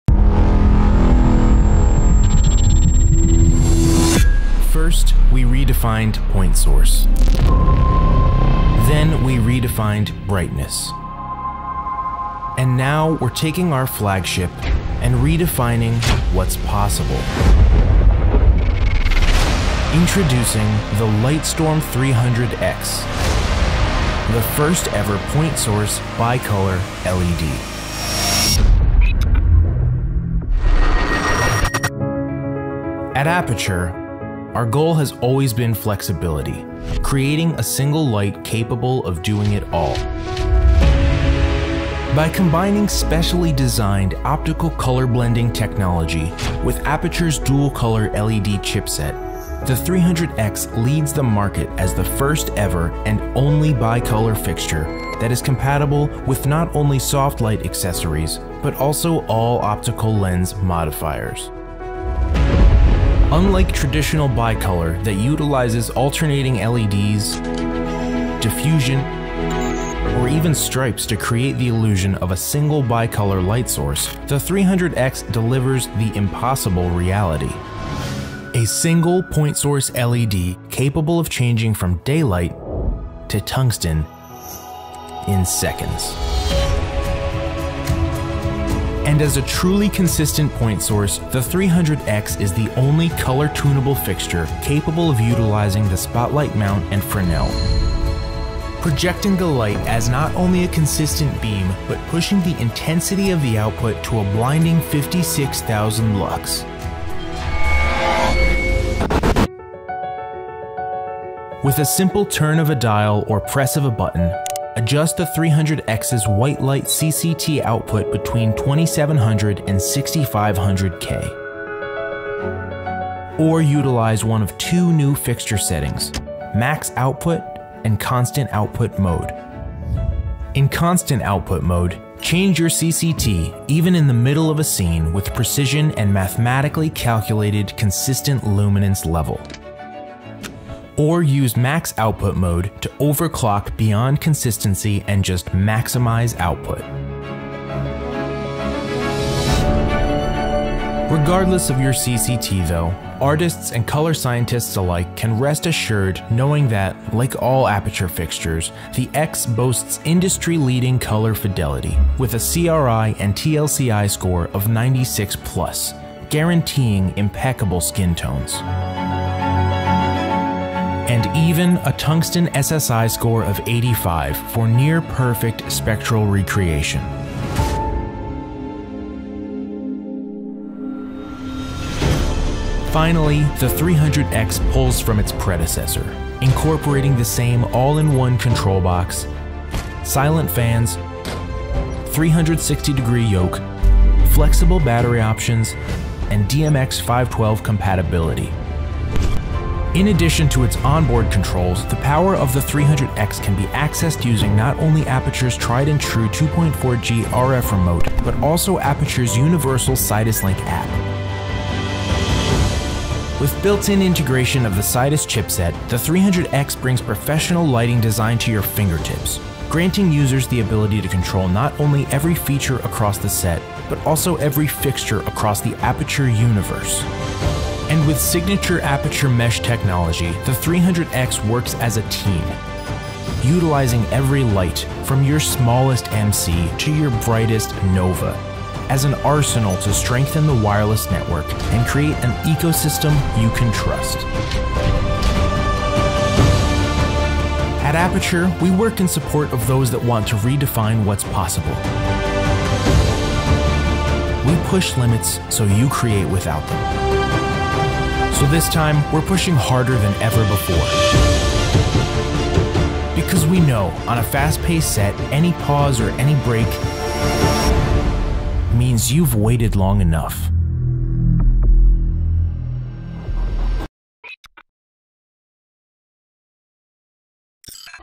방송조명 ㅣ 최고의 바이컬러 조명 어퓨쳐 라이트 스톰 300X
300X는 업계를 선도하는 바이컬러 포인트소스 조명으로  기존의 바이컬러의 색상 범위를 넘어 2700K 부터 6500K 까지 확장된 CCT의 범위를 가지며
CRI와 TLCI가 96이상, 텅스텐 SSI는 85이상, D55 SSI는 74로 업계 최고 수준을 자랑합니다.

360도 회전가능한 요크로 어떤 위치에서 조명 고정과 무음의 콘트롤 박스로 소음의 걱정없이 사용가능합니다.